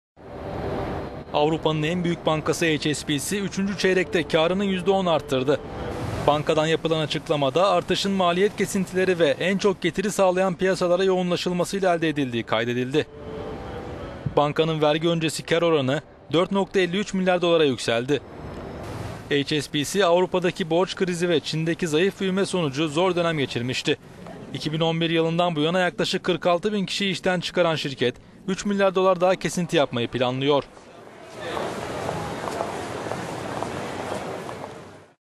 HSBC'nin karı yüzde 10 arttı - economy
Avrupa'nın en büyük bankası HSBC üçüncü çeyrekte karını yüzde 10 arttırdı. Bankadan...

Avrupa'nın en büyük bankası HSBC üçüncü çeyrekte karını yüzde 10 arttırdı. Bankadan yapılan açıklamada artışın maliyet kesintileri ve en çok getiri sağlayan piyasalara yoğunlaşılmasıyla elde edildiği kaydedildi. Bankanın vergi öncesi kar oranı 4.53 milyar Dolar'a yükseldi. HSBC, Avrupa'daki borç krizi ve Çin'deki zayıf büyüme sonucu zor dönem geçirmişti. 2011 yılından bu yana yaklaşık 46 bin kişiyi işten çıkaran şirket, 3 milyar Dolar daha kesinti yapmayı planlıyor.